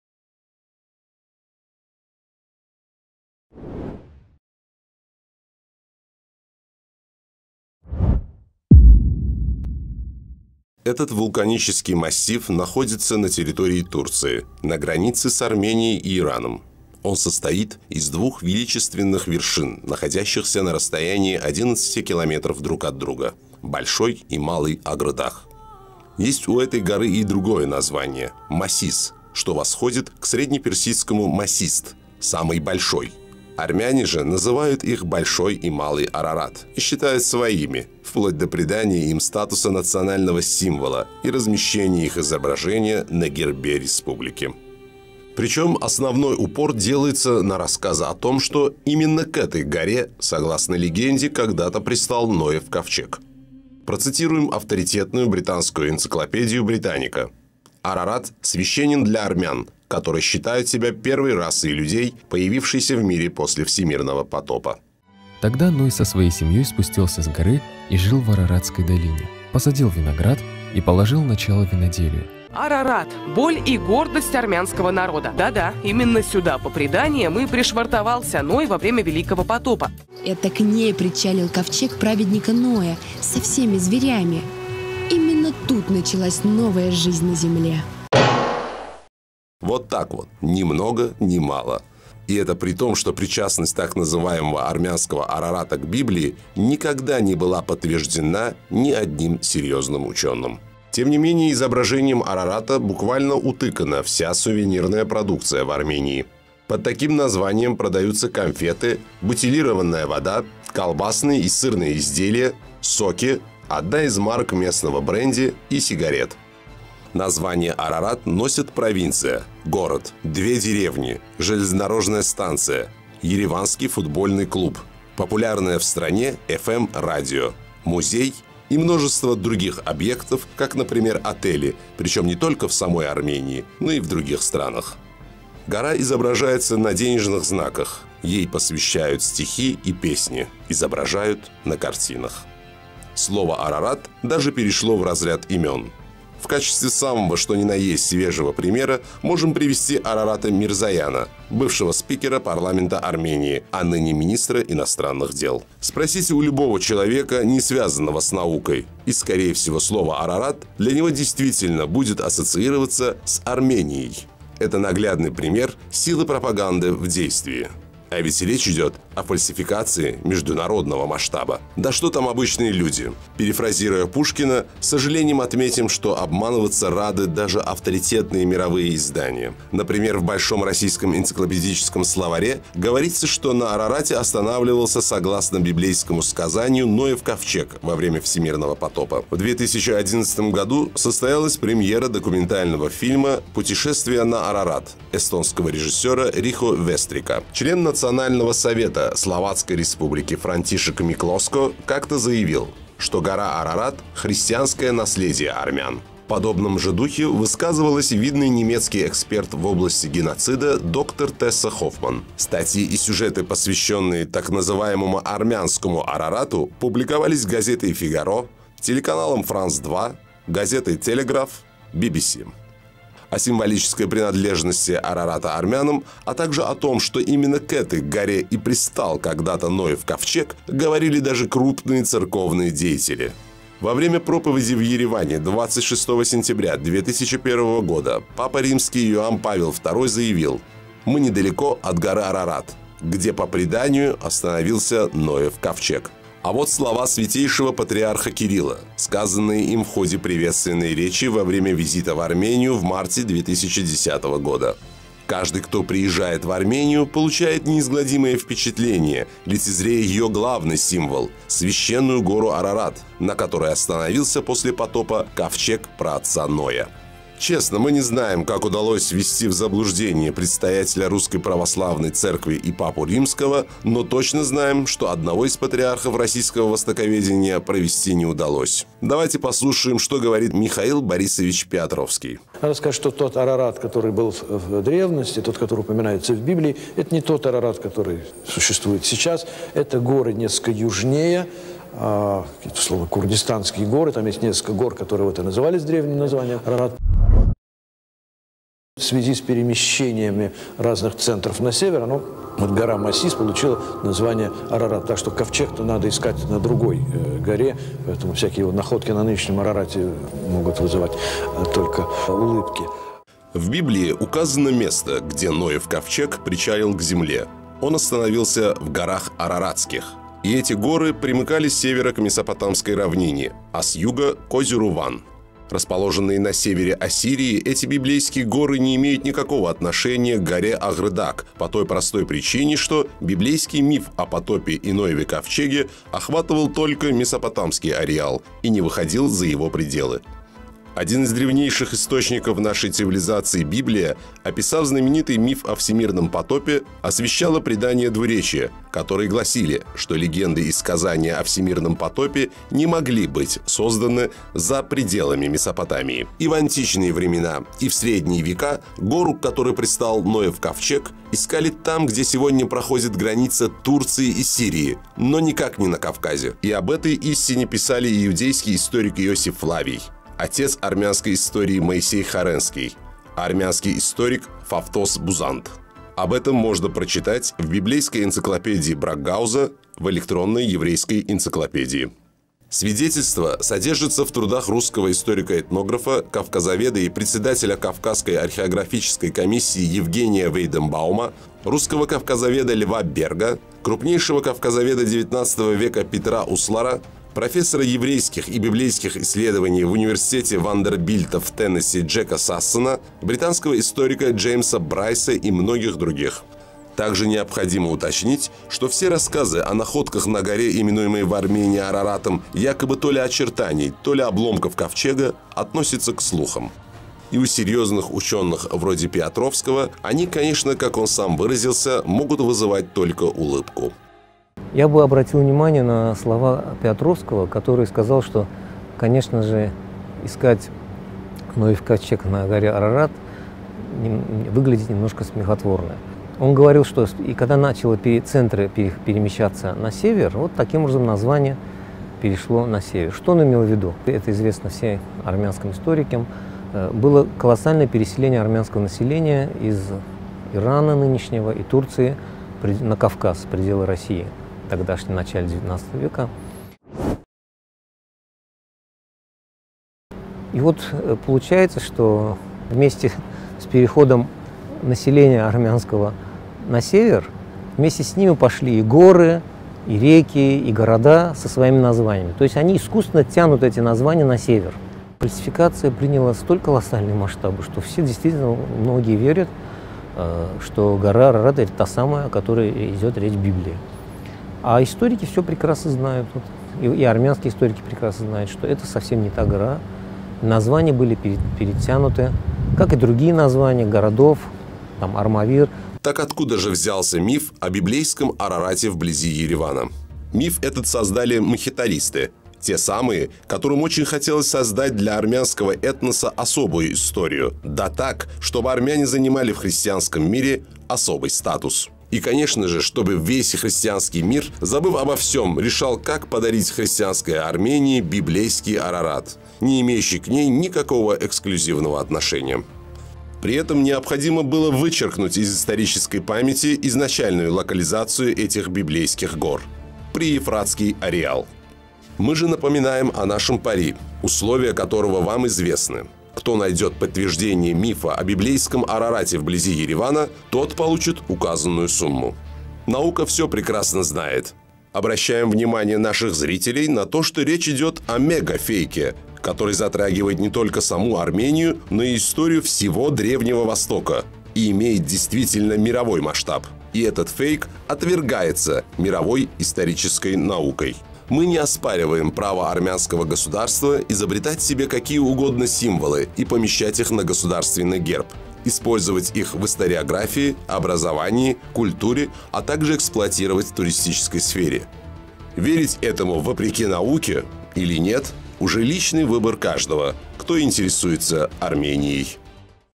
Мегафейк: "Библейский Арарат вблизи Еревана"
Речь идет о мегафейке, который затрагивает не только саму Армению, но историю Древнего Востока и имеет действительно мировой масштаб.
Мы не оспариваем право армянского государства изобретать себе какие угодно символы и помещать их на государственный герб, использовать их в историографии, образовании, культуре, а также эксплуатировать в туристической сфере.
Верить этому или нет, вопреки науке - уже личный выбор каждого, кто интересуется Арменией. А мы напоминаем о нашем пари, условия которого вам известны - кто найдет подтверждение мифа о «библейском Арарате вблизи Еревана» - тот получит указанную сумму.

*Древнееврейский историк Иосиф Флавий в своей книге Иудейские древности ссылаясь на халдеянина Бероса писал о потопе: «Говорят, что еще до сих пор ковчег сохранился на горе Кордуйской» В примечаниях этой книги в 33-й ссылке подчеркивается: «по сирийскому преданию, ковчег остановился на хребте Джебель Чуди, сирийское Карду, недалеко от озера Ван».
**Моисей Хоренский в своей «Истории» относил этот ороним к Приевфратскому ареалу. И подчеркивал, что эта библейская гора сирийцами называлась Сарарадом. (История Армении Моисея Хоренского.  Прим.45, стр. 225, 226)
***Армянский историк V века Фавтос Бузанд в своей «Истории» писал: «Гора Сарарад локализовалась в области Кордук. ( Фавтос Бузанд. «История» кн.3, гл.10.)
Русский этнограф, историк, кавказовед и председатель Кавказской археографической комиссии, тайный советник Евгений Густавович Вейденбаум, ссылаясь на первоисточники писал, что эта библейская гора находилась южнее оз. Ван, и Ветхий Завет, описывая плавание ковчега, имел в виду ороним в области Кордук, а не вершину, которая у блуждающих армян «всегда называлась горою Масис» (Вейденбаум Е.Г.Большой Арарат и попытки восхождения на его вершину. Тифлис, 1884. С.130.).
Русский географ-энциклопедист, историк географии Л.Берг в своей статье «Арарат», напечатанной в Энциклопедическом словаре Русского Библиографического Института Бр. А. и И. Гранат, также относил локализацию горы Арарат к приевфратскому хорониму и особо отмечал, что «под страною Арарат древние евреи подразумевали местность к северу от равнин Месопотамии, т.е. горы к югу от оз. Ван» (Энциклопедический словарь русского библиографического института гранат. Под редакцией проф. Ю.С. Гамбарова, проф. В.Я. Железнова, проф. М.М. Ковалевского и др. Том. Третий. Москва, 1922. С.378.)
Пётр Карлович Услар, один из крупнейших кавказоведов XIX века, Член Кавказского отдела Императорского Русского географического общества, член-корреспондент по разряду лингвистики Историко-филологического отделения Академии наук, ссылаясь на Ветхий Завет писал, что еврейская легенда о всемирном потопе «говорит не о горе Арарат, а о горах Араратских», которые были расположены, как подразумевали древние евреи на высокой стране, лежащей «к северу от равнин Месопотамии, т.е. отрасли Тавра к югу от озера Ван»( Услар П.К. Древнейшие сказания о Кавказе. В Сборнике сведений о кавказских горцах, т. Х, с.17.)